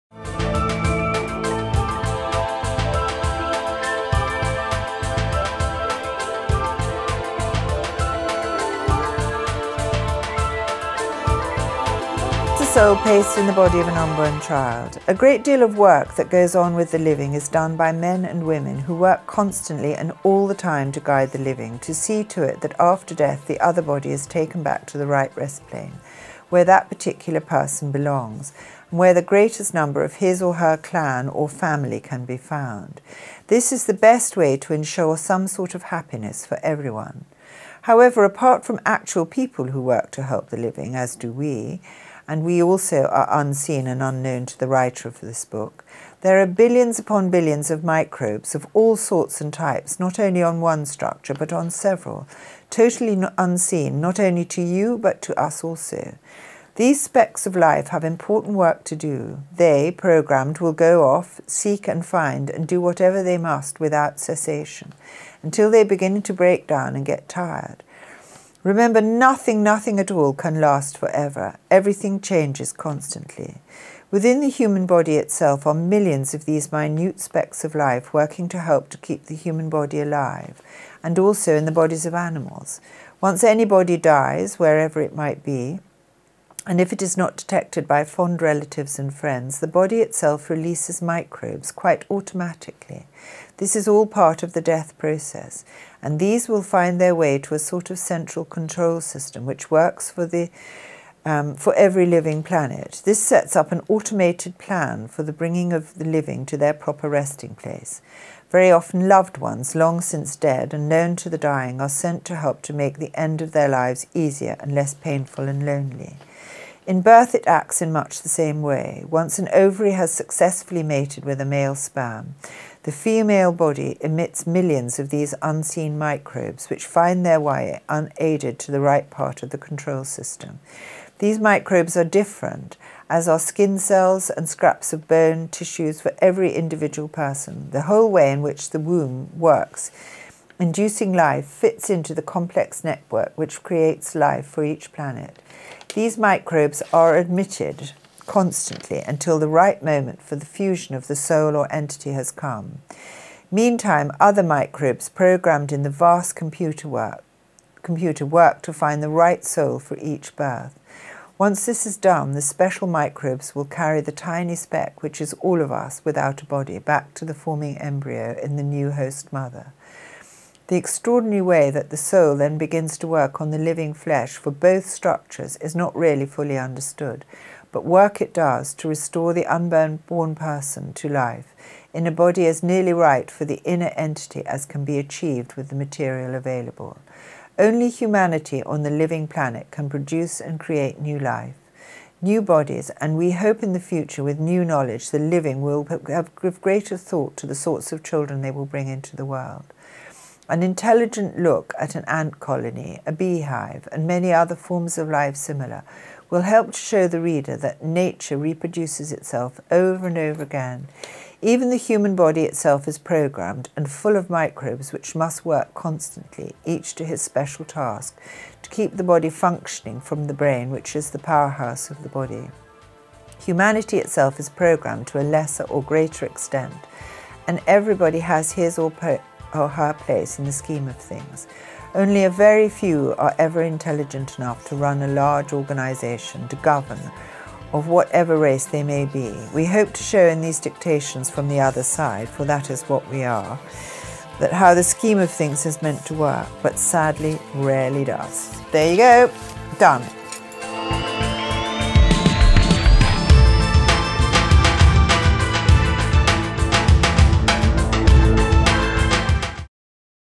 Message for Humanity by Jane Shattock Session 3 part 6
Session 3 of the continued series "We are What We Are" read by Jane Shattock. Her mother was "visited", by people from 'upstairs' with 'a message for humanity'.  This is in-depth message, much of it very politically incorrect, in today's stark work of censorship. Its read as received. It is not the views of Jane, her mother or the Bases project.  Jane suffers from some of the issues explained, and has done so to this day.
This a message, don't shoot the messenger.